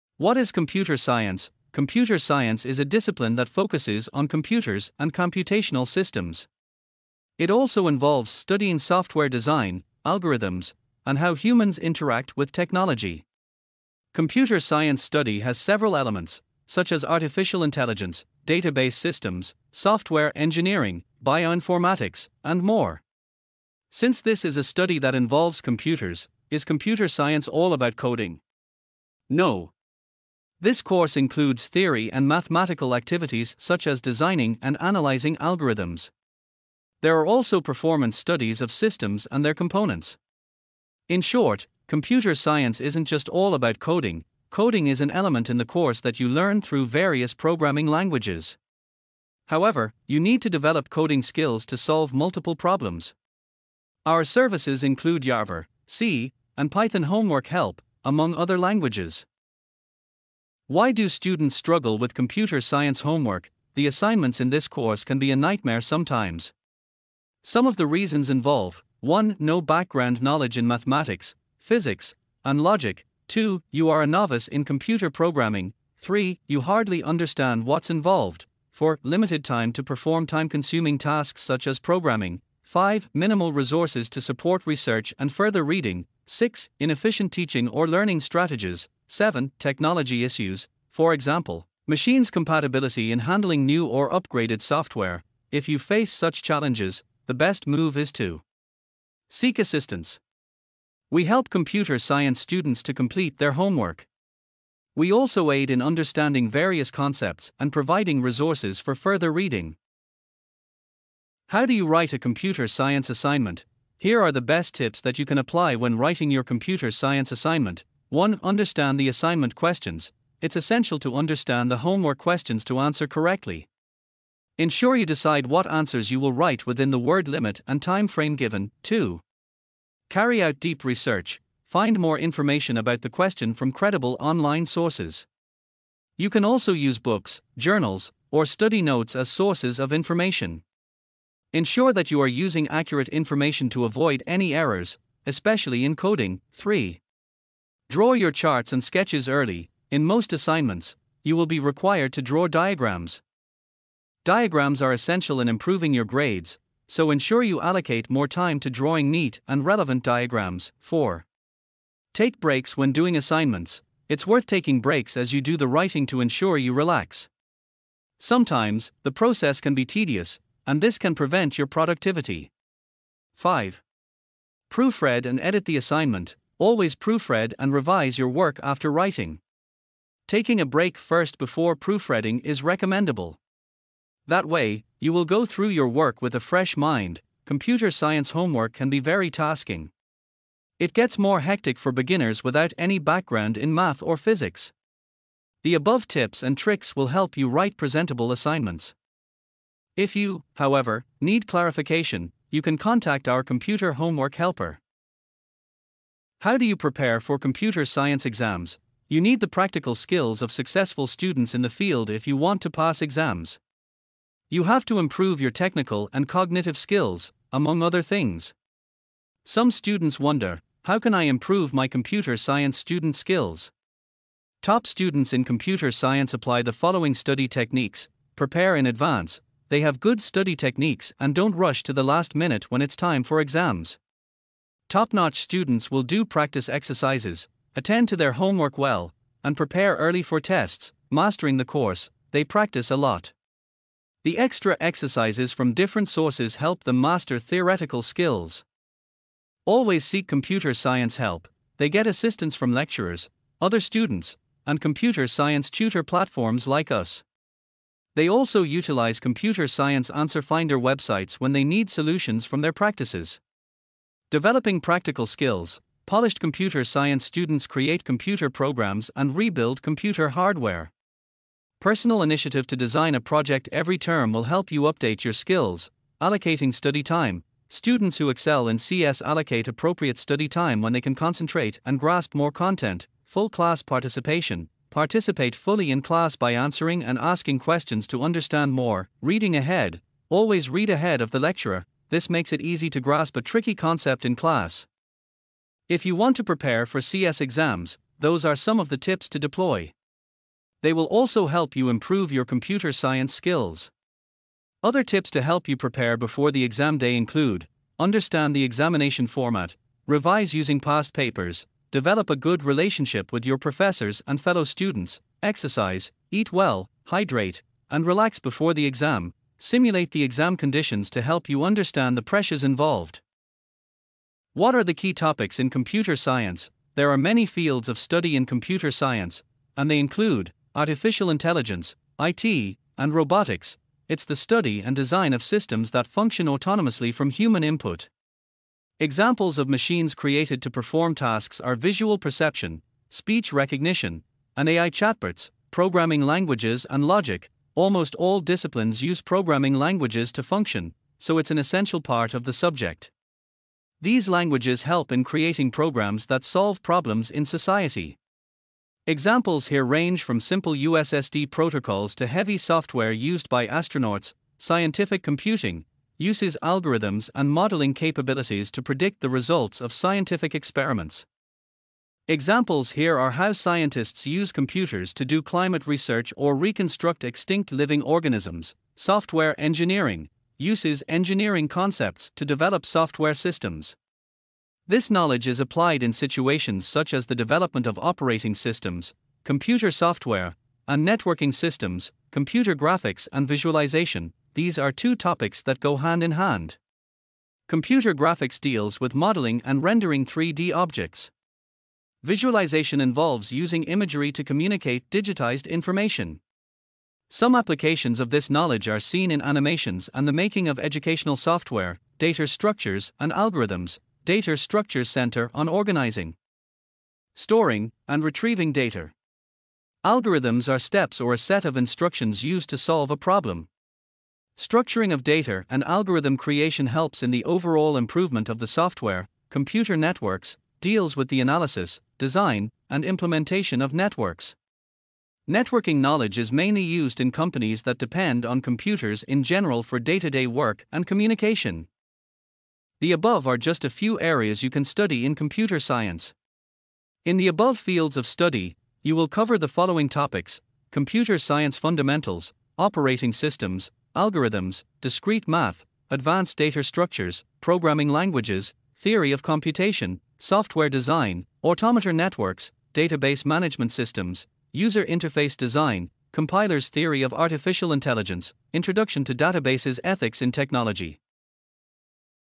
5 Tips on How to Write a Computer Science Assignment.
Check out the updated version of 5 Tips on How to Write a Computer Science Assignment.📝✍🏻

In today's video, we're diving into the world of computer science assignments and equipping you with 5 invaluable tips to help you write stellar and impactful projects. Whether you're a seasoned computer science student or just starting your journey, these tips will elevate your assignment game to new heights!

To get direct to the part you are more interested on follow the time code below🕐:
00:00:00 Intro
00:01:01 Challenges
00:01:51 How to Write Computer Science Assignment
00:03:26 How to prepare for computer science exam
00:05:24 Key Computer Science topics